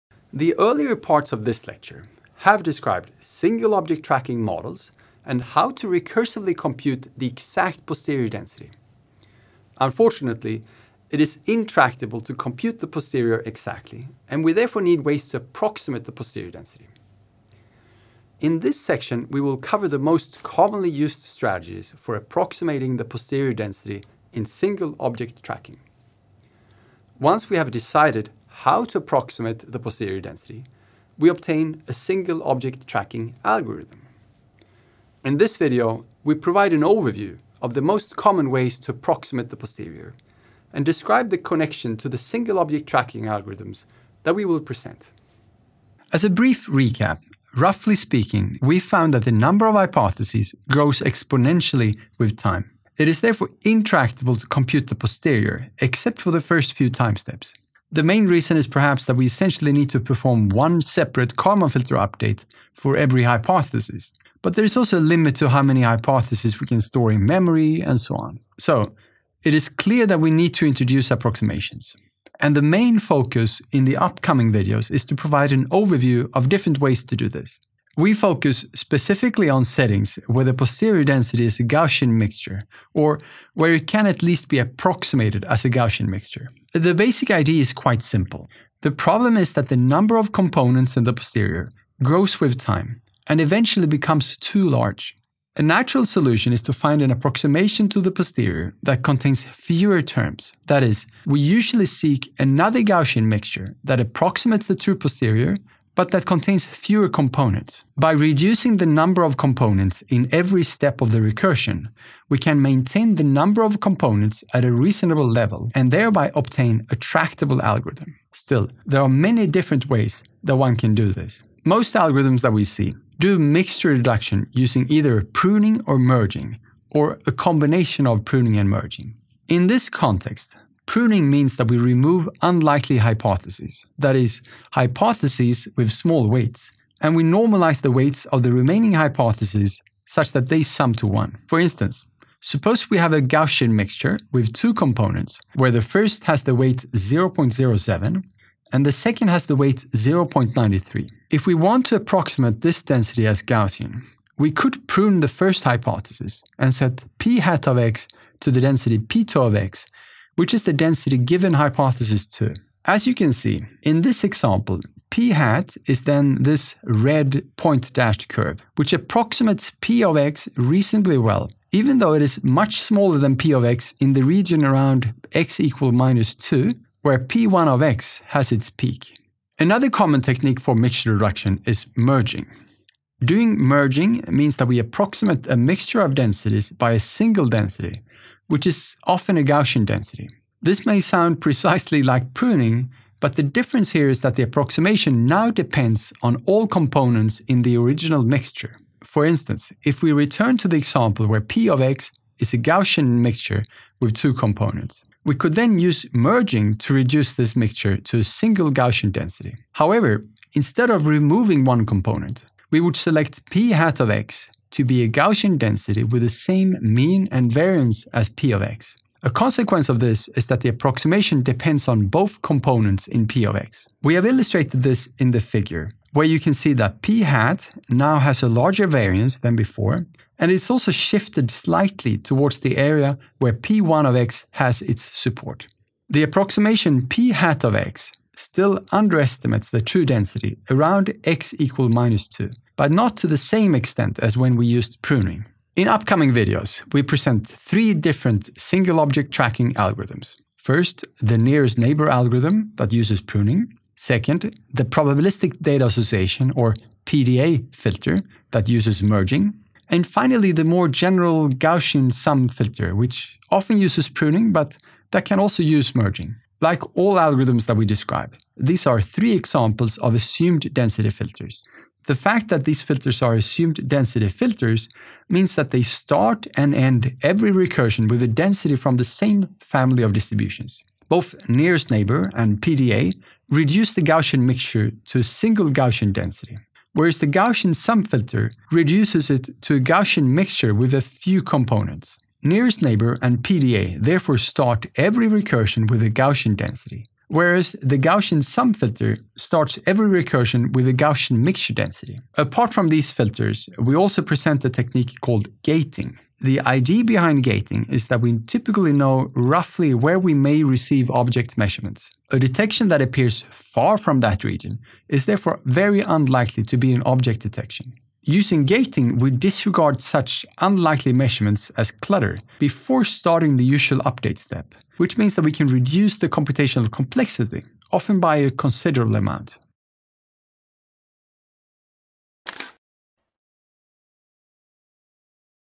An Overview of SOT Algorithms
This video is part of a lecture series about Multiple Object Tracking. It has six parts,

The course is offered as a Massive Open Online Course (MOOC) on edX. This includes the above video lectures, as well as quizzes and home assignments where you implement tracking filters,